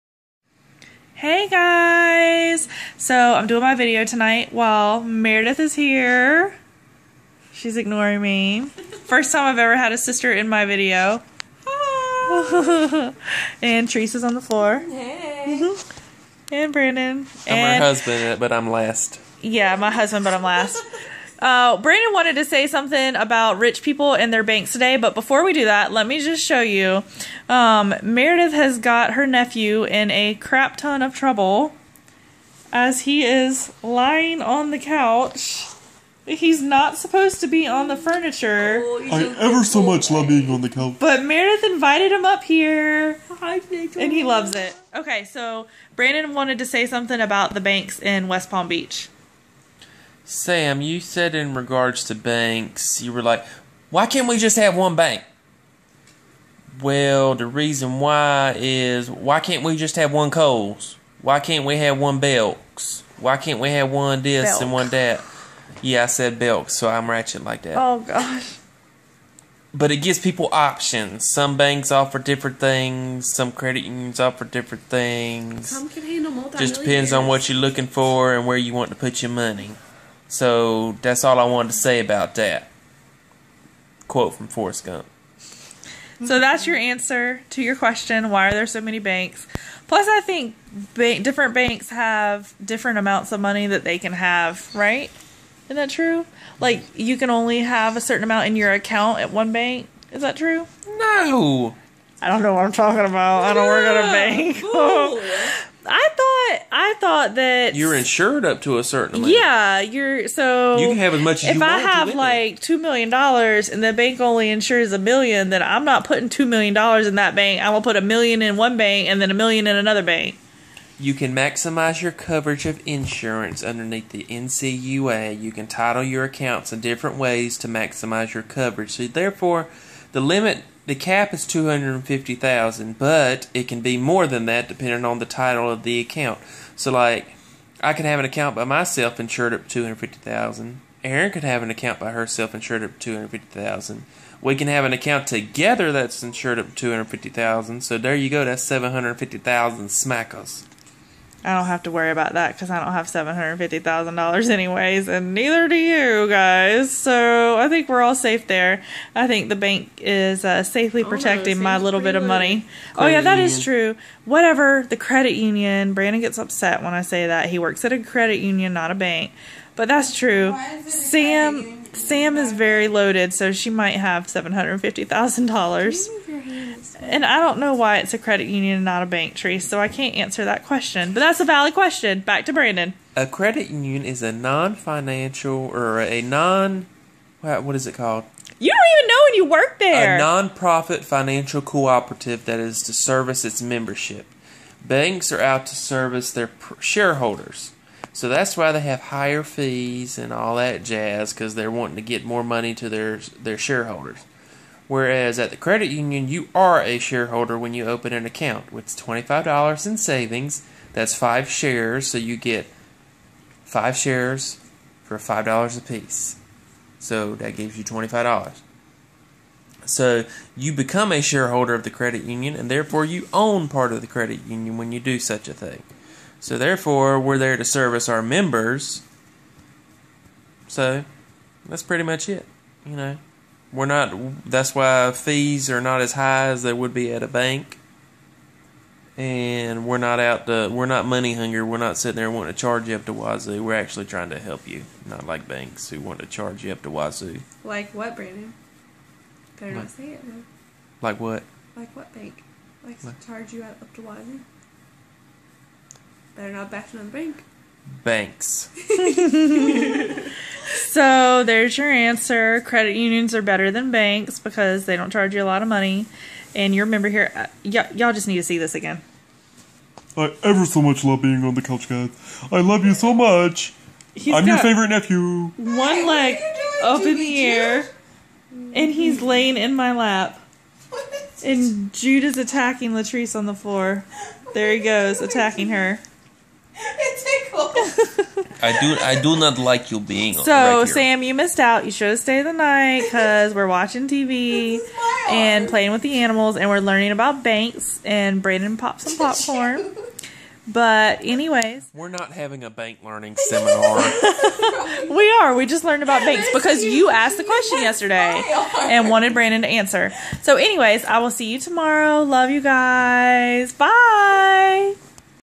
Bank lesson and Fletcher breaking the rules!
Brandon educates us on money and Meredith let Fletcher break the rules and jump on the couch.